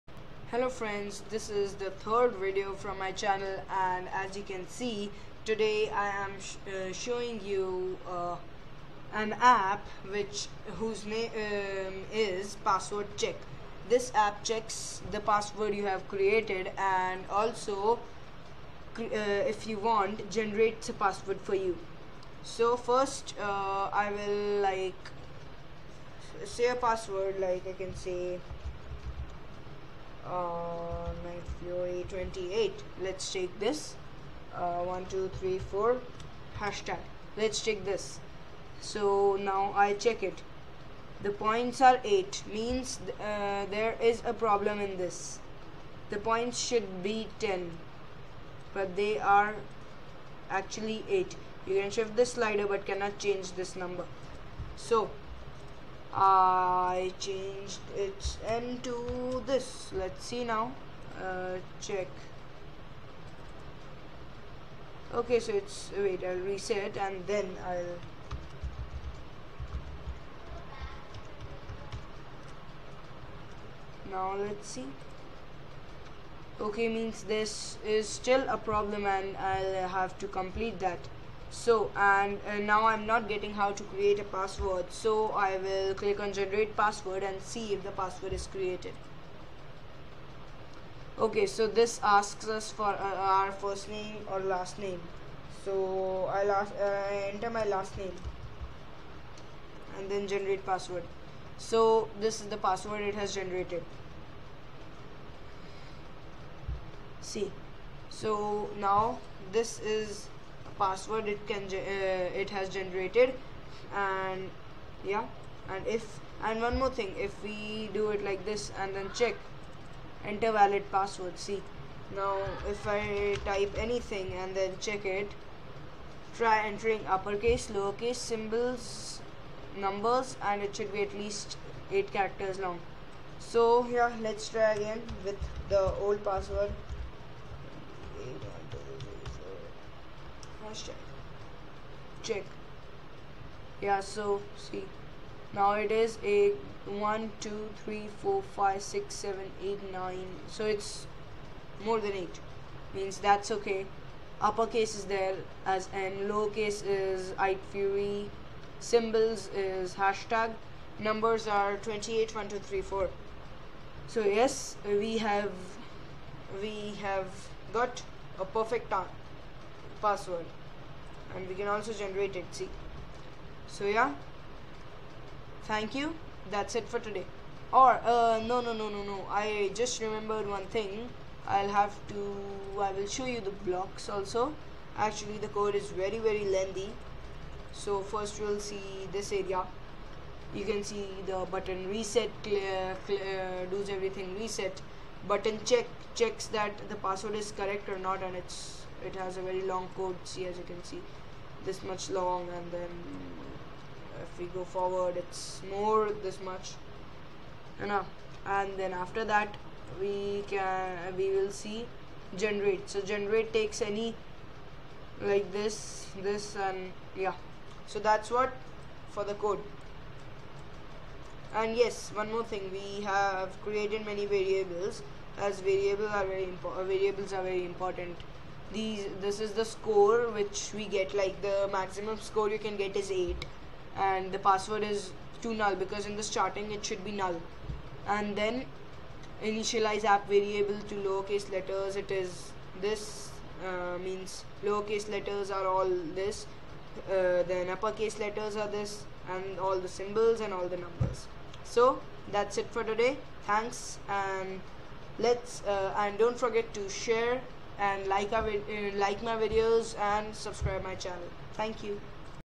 Password Check App | PA Coding
This app helps you create your password or generate it itself.